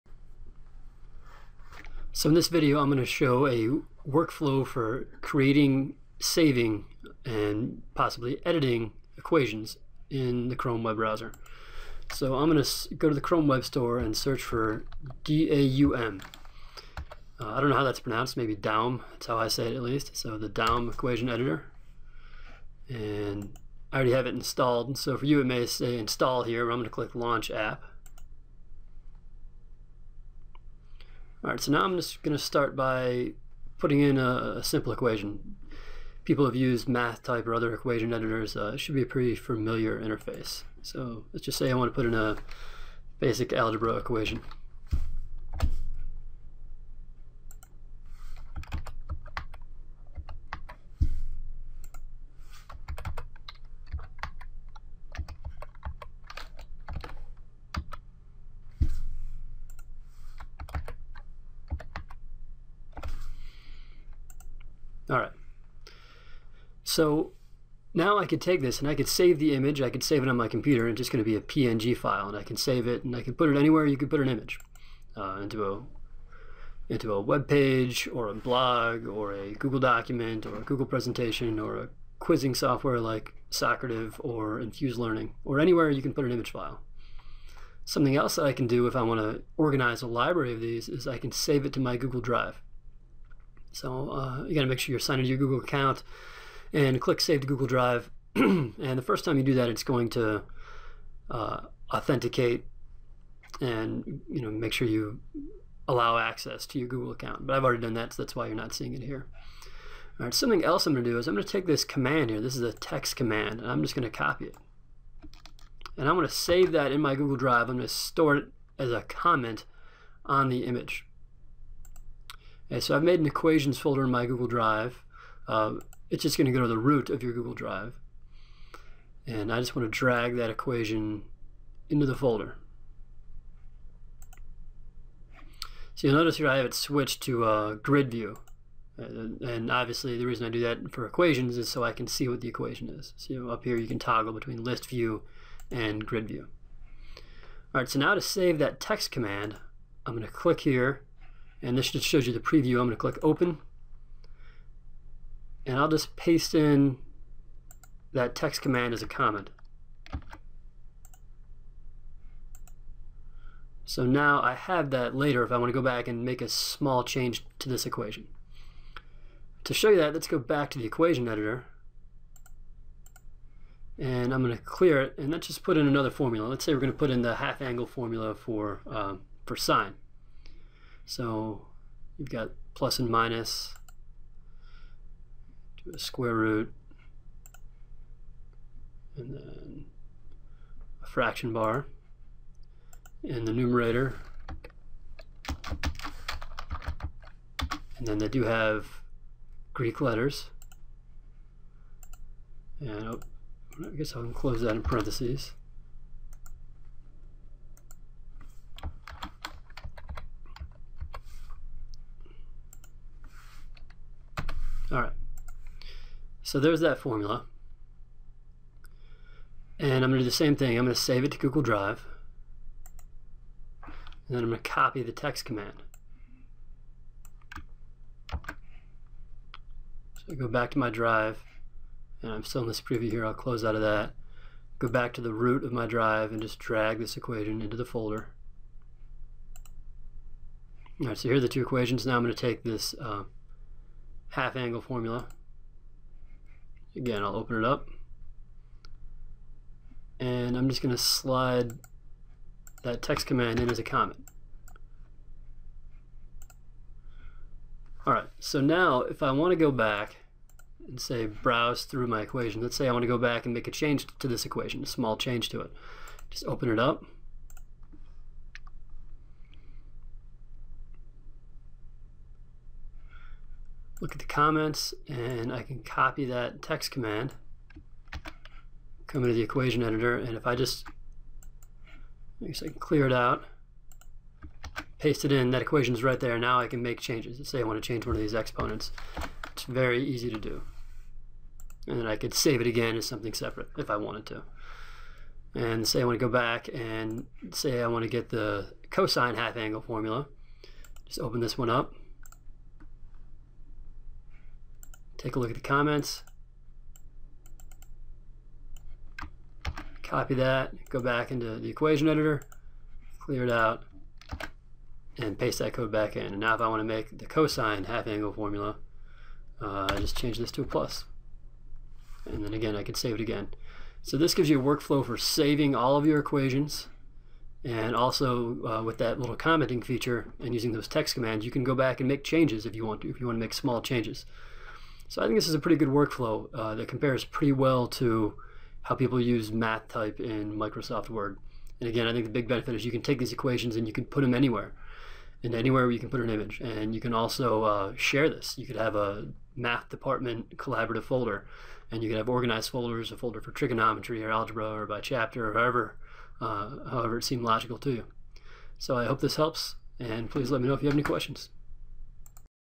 Daum Equation Editor Saving and Editing Later
In this video I show how to save Daum Equations to your Google Drive, then store the TeX code as a comment so that you can go back and modify it later.